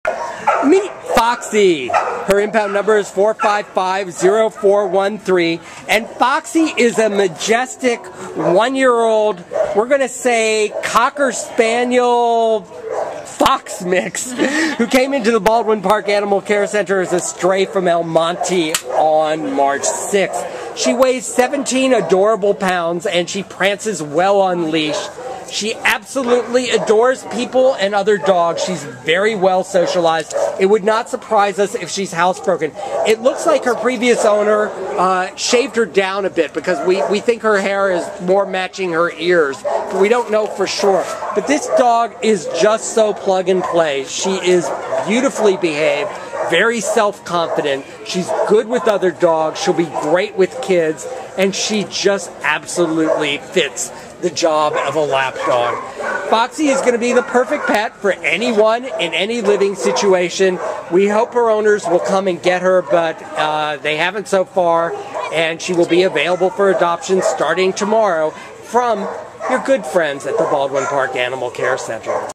Foxy A4550413 ADOPTED!!!
Foxy is a majestic one year old red and brown female Spaniel mix puppy who was found in El Monte on March 6th and brought to the Baldwin Park Animal Care Center.  Weighing seventeen pounds, Foxy prances beautifully on leash and we think she is housebroken.  She is sociable with other dogs and we think she will be fine with children.  Foxy is a confident puppy who loves attention.  Foxy is the perfect indoor pet for anyone in any living situation at all.